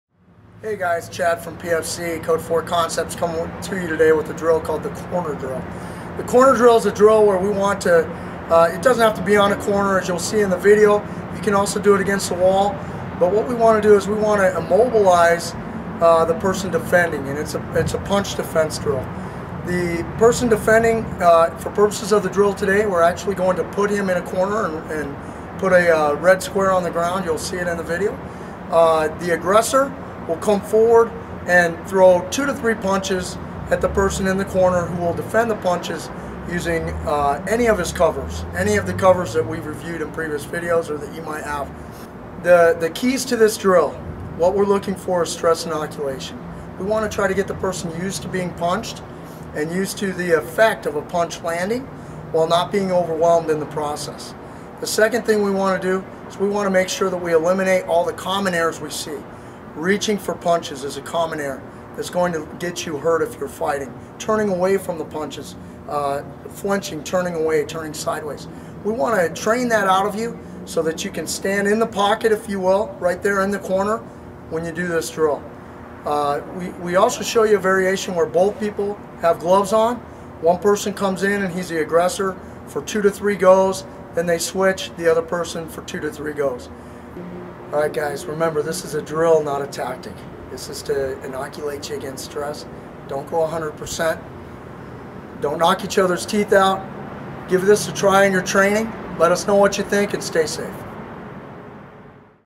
PFCtraining.com - Code 4 Concepts "Corner Drill"
Chief of Code 4 combatives, Chad Lyman, breaks down a simple but effective stress inoculation drill.